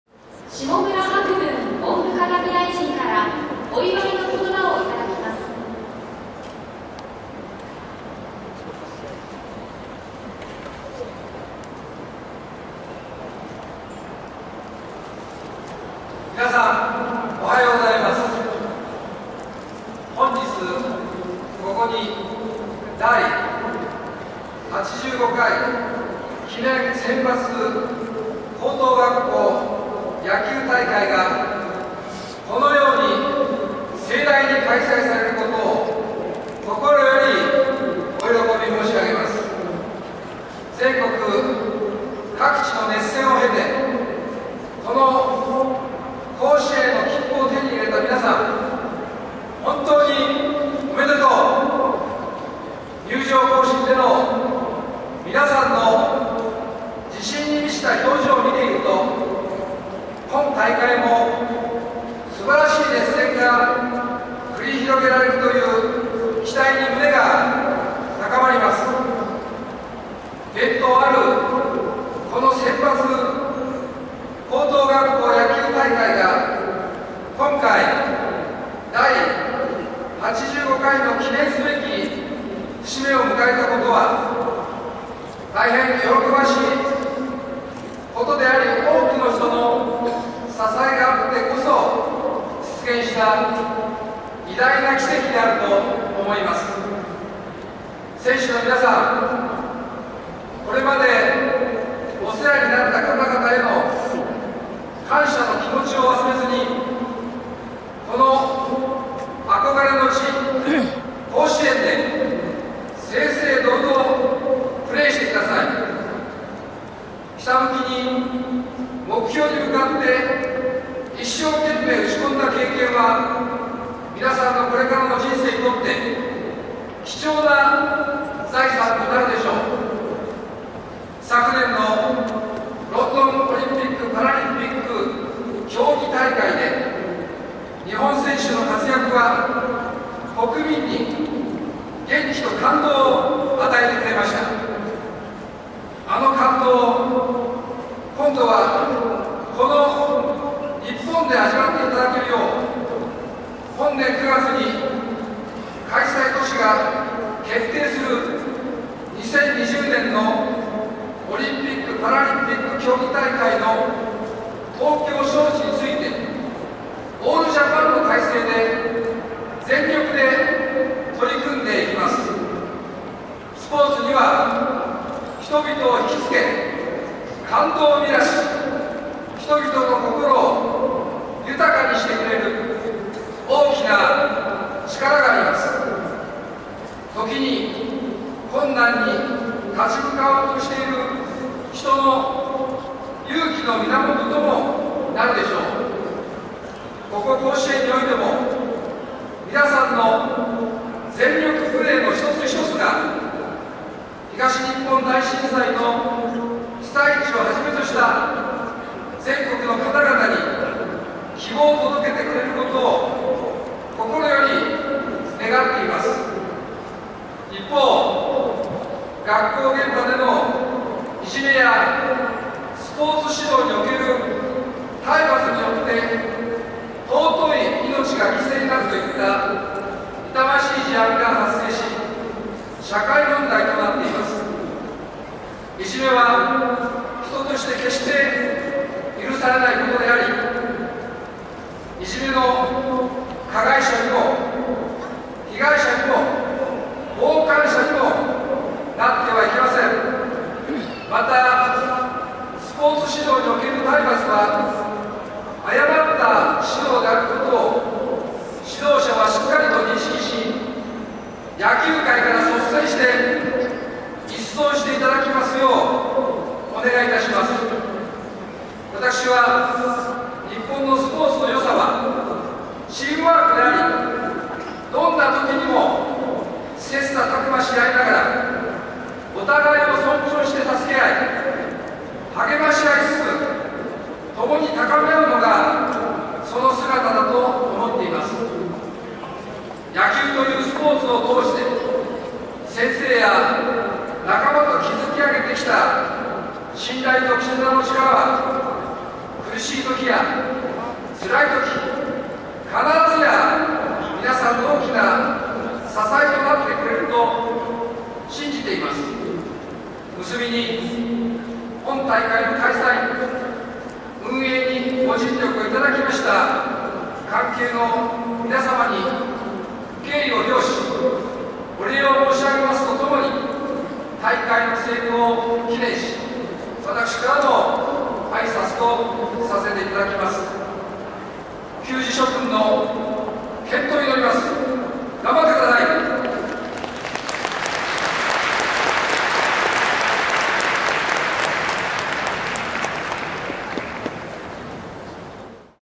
第85回センバツ開会式・下村博文 文部科学大臣のスピーチ
平成25年　第85回記念選抜高等学校野球大会＠阪神甲子園球場
下村博文氏は母校の高高（群馬県立高崎高校）が出場した昨年に来賓として出席できなかったのが残念と言うほかありません。
CAM:GC-PX1